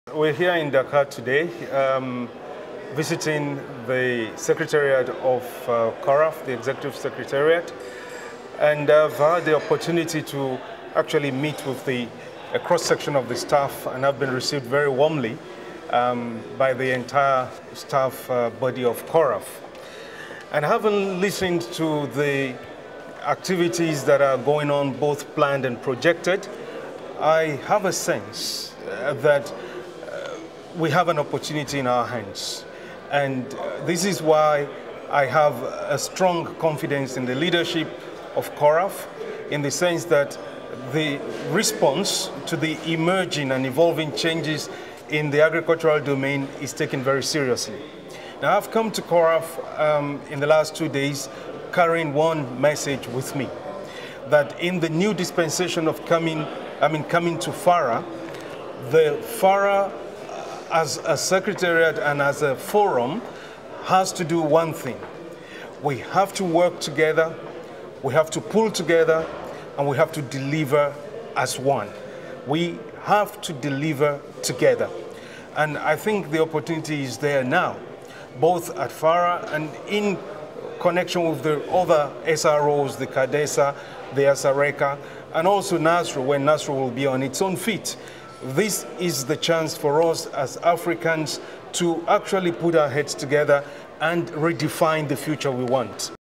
ITW DR YEMI AKIMBAMIDJO OK
Dr Yémi AKimbamidjo, le nouveau Directeur Exécutif du Forum pour la recherche agricole en Afrique, a visité le Secrétariat Exécutif du CORAF/WECARD, une des constituantes du FARA, le vendredi 17 Mai 2013.